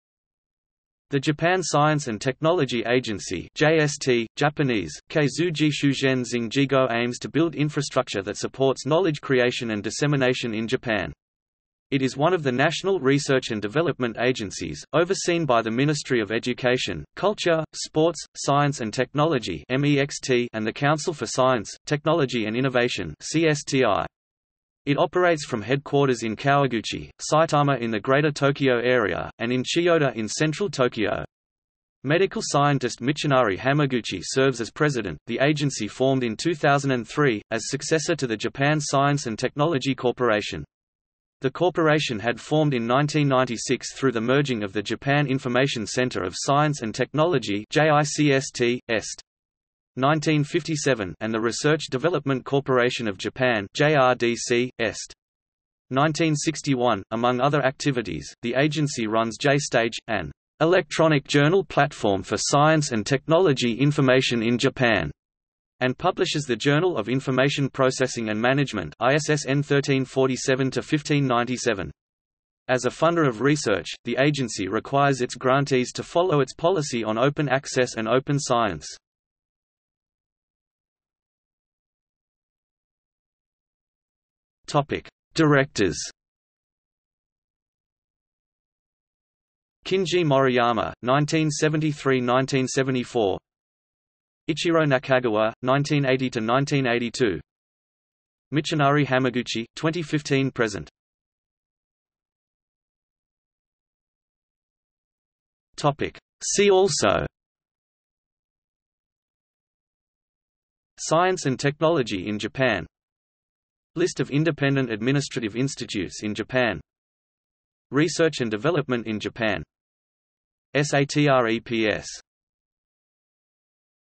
Japan Science and Technology Corporation | Wikipedia audio article
00:01:20 1 Directors
00:01:41 2 See also

- Socrates

SUMMARY
The Japan Science and Technology Agency (JST; Japanese: 科学技術振興機構) aims to build infrastructure that supports knowledge creation and dissemination in Japan. It is one of the National Research and Development Agencies, overseen by the Ministry of Education, Culture, Sports, Science and Technology (MEXT) and the Council for Science, Technology and Innovation (CSTI).  It operates from headquarters in Kawaguchi, Saitama in the Greater Tokyo Area, and in Chiyoda in central Tokyo. Medical scientist Michinari Hamaguchi serves as president.The agency formed in 2003, as successor to the Japan Science and Technology Corporation. The corporation had formed in 1996 through the merging of the Japan Information Center of Science and Technology (JICST, est. 1957) and the Research Development Corporation of Japan (JRDC, est. 1961).Among other activities, the agency runs J-STAGE, an "electronic journal platform for science and technology information in Japan," and publishes the Journal of Information Processing and Management (ISSN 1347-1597). As a funder of research, the agency requires its grantees to follow its policy on open access and open science.